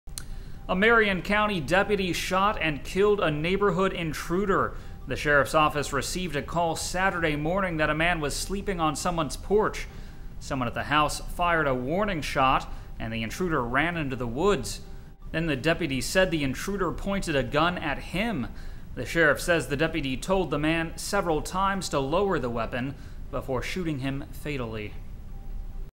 Deputy shoots and kills neighborhood intruder in Jasper TN
A Marion County deputy shot and killed a neighborhood intruder.

The sheriff's office received a call Saturday morning that a man was sleeping on someone's porch.

Someone at the house fired a warning shot and the intruder ran into the woods.

Then the deputy said the intruder pointed a gun at him.

The sheriff says the deputy told the man several times to lower the weapon before shooting him fatally.